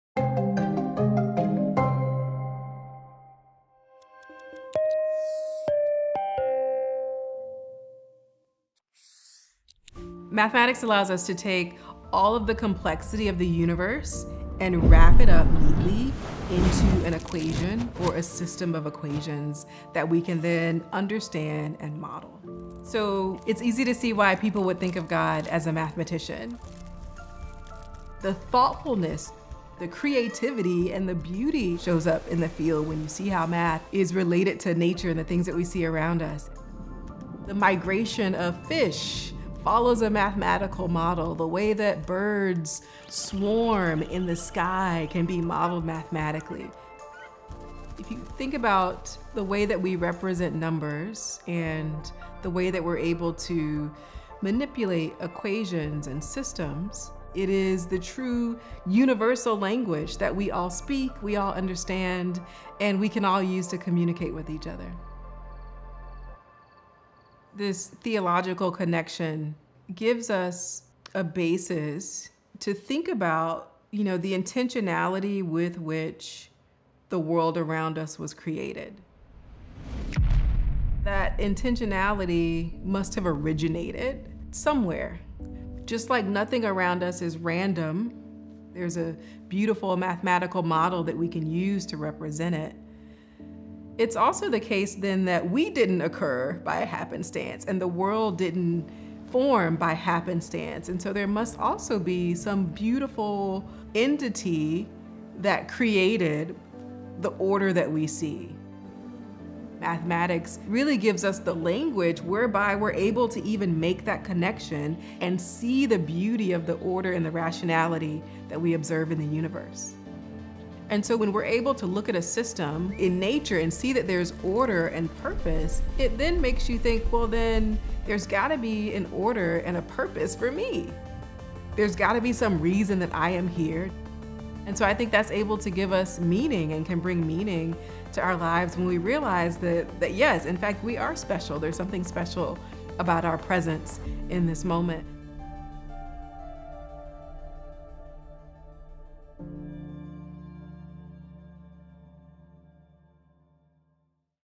How math brings order to our universe | Talithia Williams
Statistician Talithia Williams on how math is the clearest path to understanding our existence.

What does math have to do with theology? According to Dr. Talithia Williams, a math professor and science communicator, quite a lot.

In just under three minutes, Williams explains how mathematics connects the natural world with deeper ideas of order and purpose. Math, she says, helps us make sense of everything from the migration of fish to the patterns we see in nature, uncovering the structure of our universe.

Dr. Williams believes math is more than just numbers—it’s a universal language that offers insights into our existence. This intersection of math, nature, and culture reveals something deep and profound about our lives and the purpose behind them.